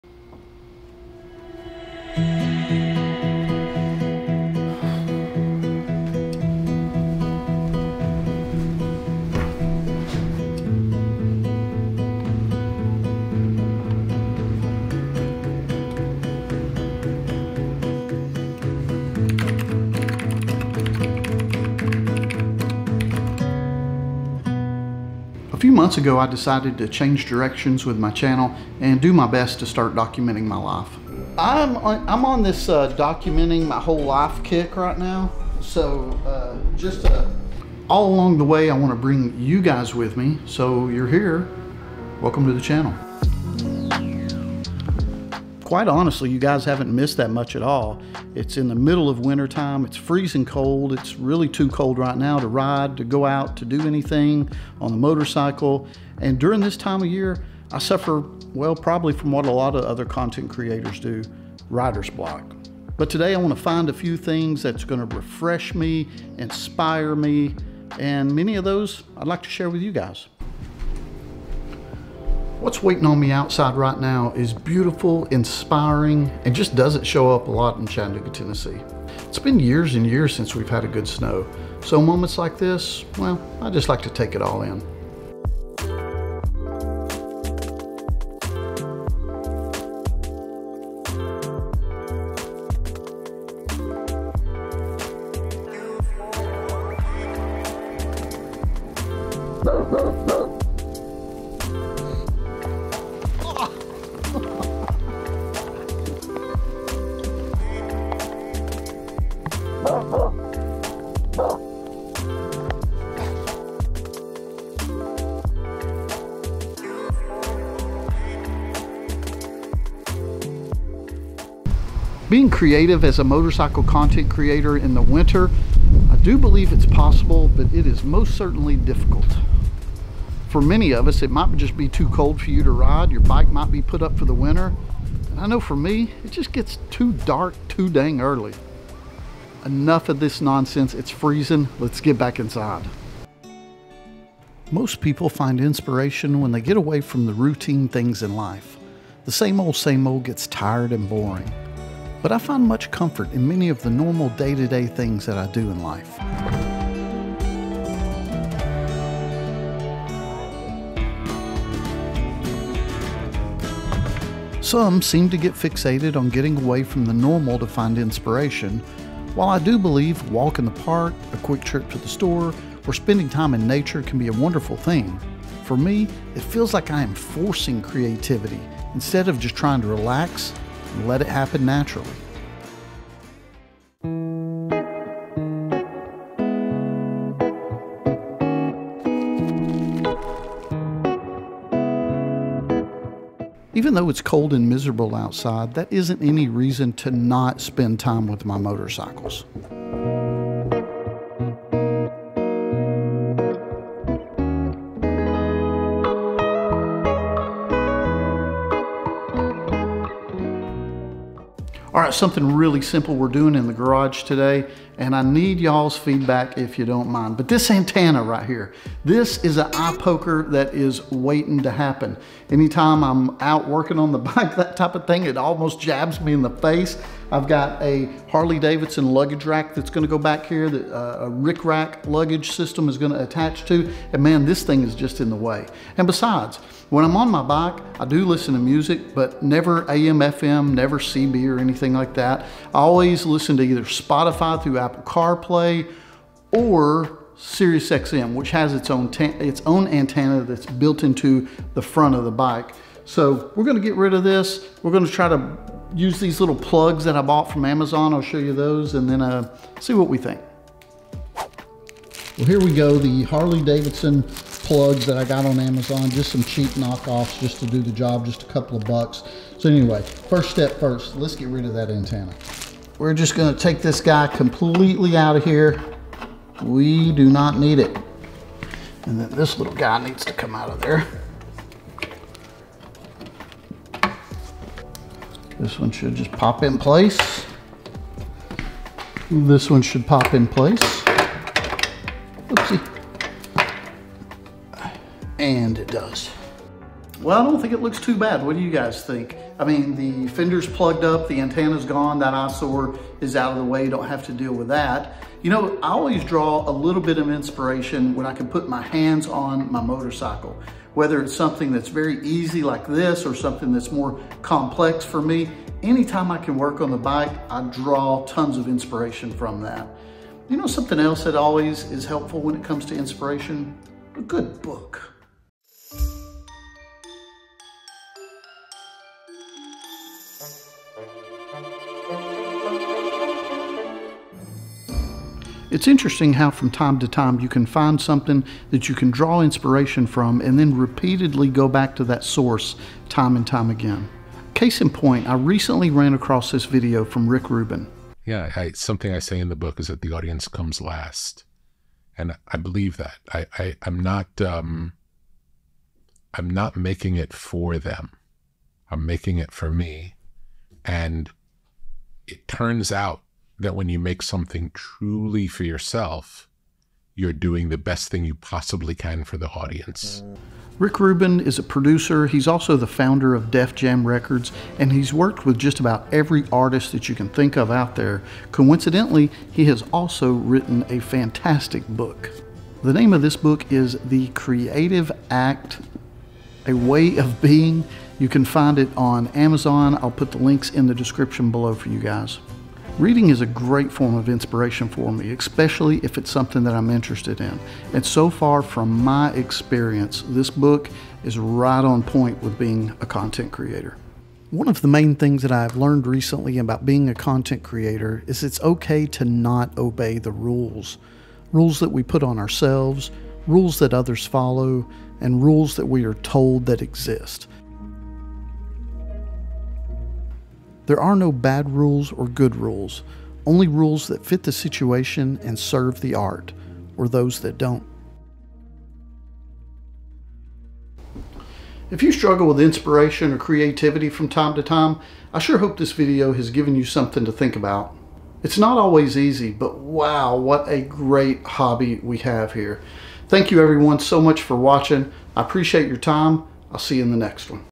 Finding Inspiration as a Motorcycle Content Creator
It's not always easy to find inspiration as a motorcycle content creator. Here are some helpful suggestions that have served me well over the past few years.

The Creative Act: A Way of Being: Rubin, Rick

⏱️TIMESTAMPS⏱️
0:00 - Intro
0:43 - Winter and Snow
2:23 - Routines
3:08 - Motorcycles
5:25 - Book Recommendation
6:44 - Closing

Please follow on YouTube, Instagram, Facebook, and TikTok!

My Motorcycles
2023 Harley Davidson Road Glide Special 120th Anniversary
2022 Harley Davidson Low Rider ST